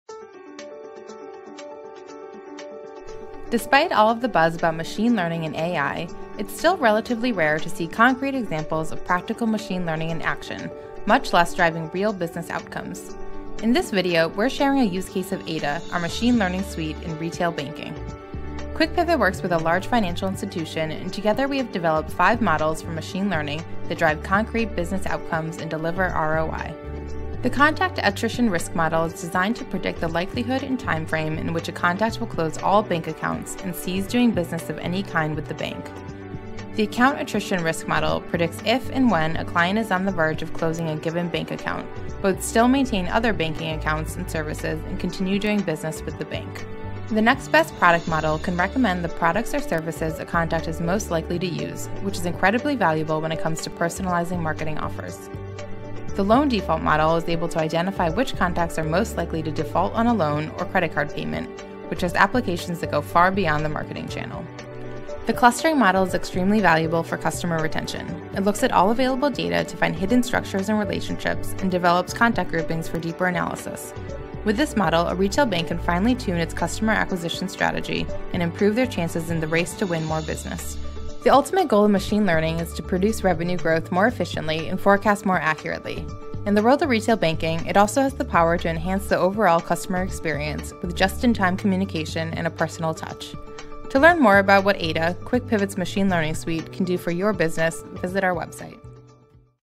Practical Machine Learning: The Use Case in Retail Banking
In this video, we're sharing a use case of Ada, our machine learning suite, in retail banking.